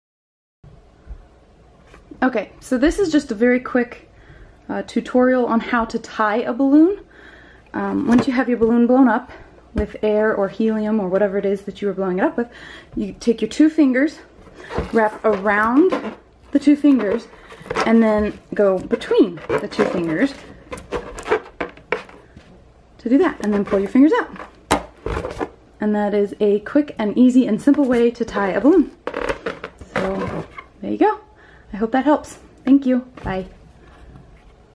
How To Tie A Balloon - Easy
Do you need to learn how to tie a balloon knot? In this short tutorial I am going to show you an easy, and quick way to tie a balloon. Tying a balloon should be simple, not complicated. Let me know you how.

Fun With Balloons!